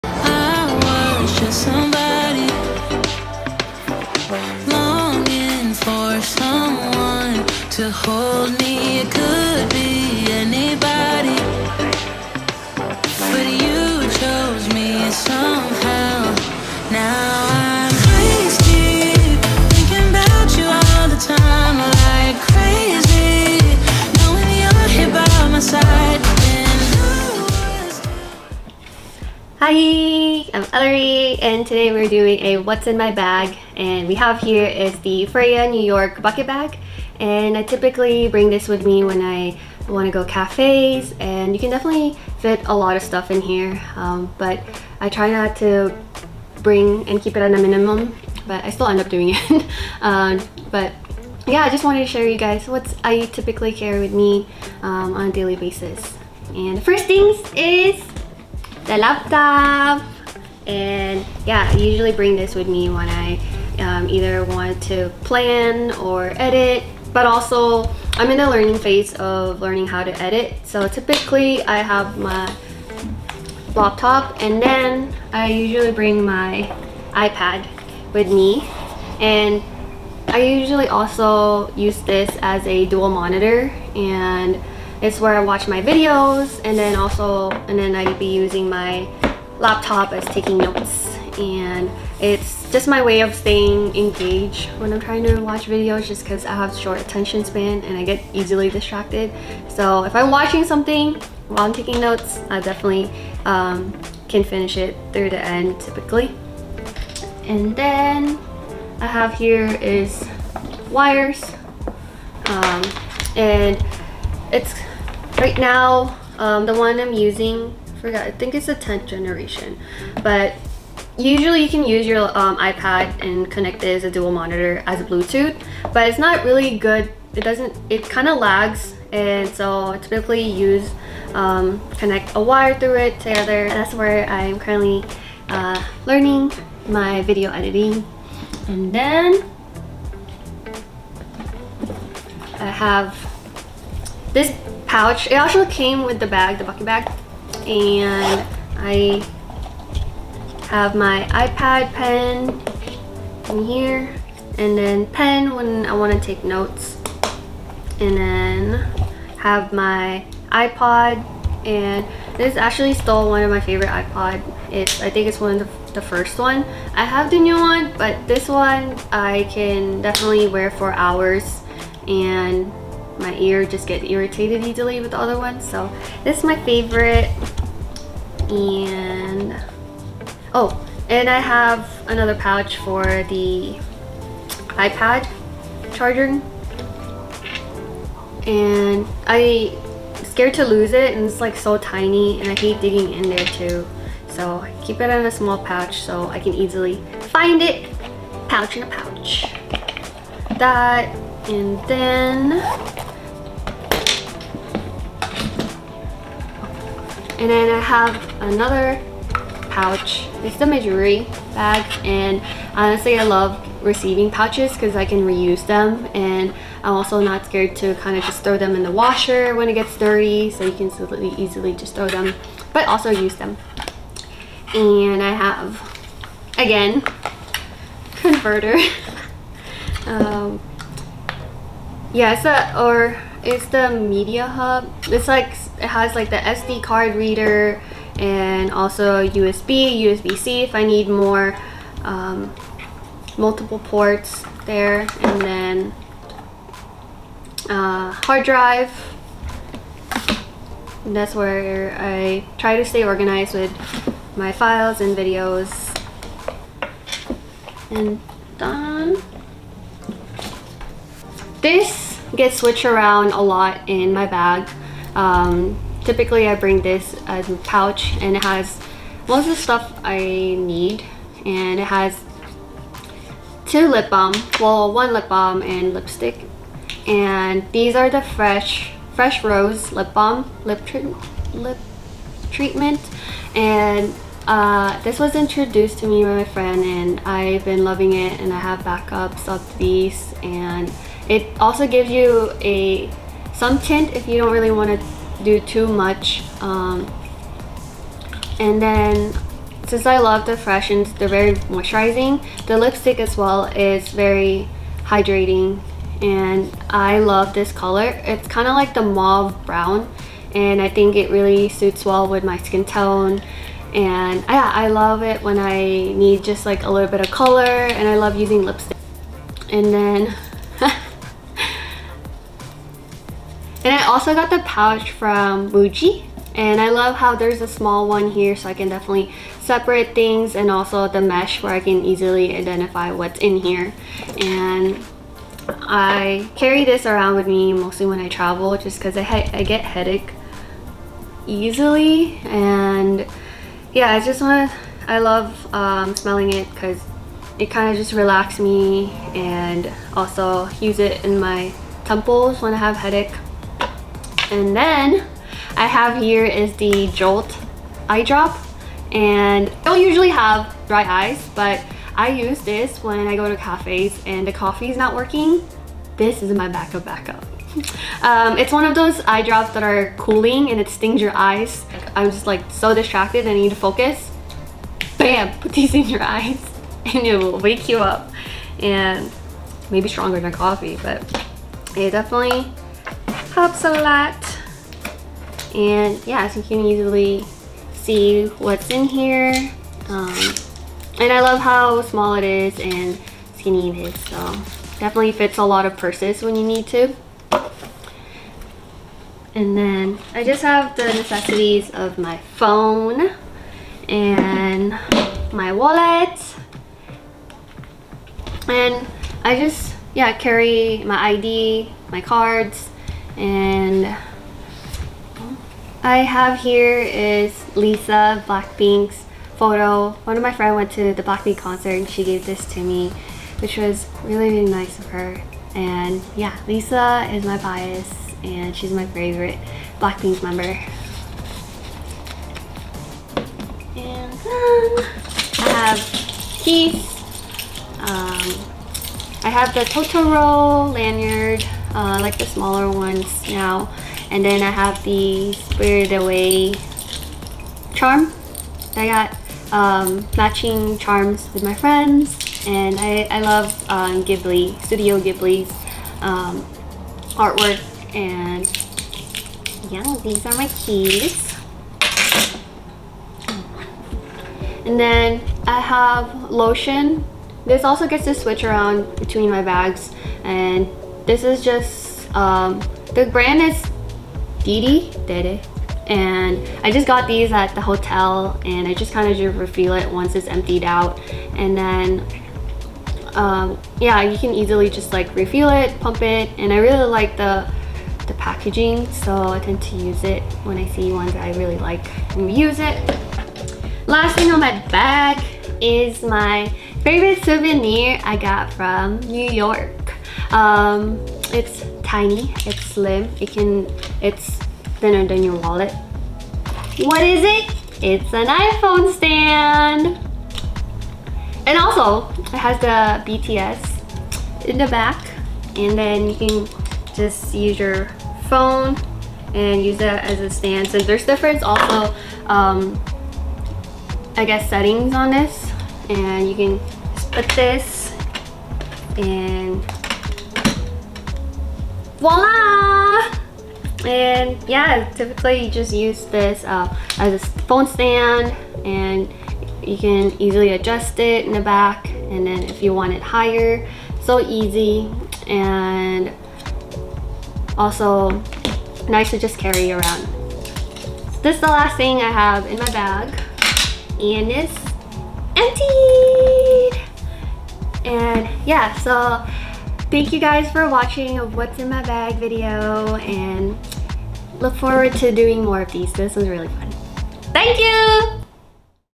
What's in my bag| Freja New York Tote Bag
Hi I'm Ellery and I'll be showing you guys what's in my bag. I've been loving this bag and I bring it with me when I travel and go to cafes. I don't really have much in my bag and try to be minimal and simple.

Cafe Location:  📍Top Pot Doughnut 2124 5th Ave Seattle

00:02 📍Top Pot Doughnut
00:38 Freja New York|Tall Tote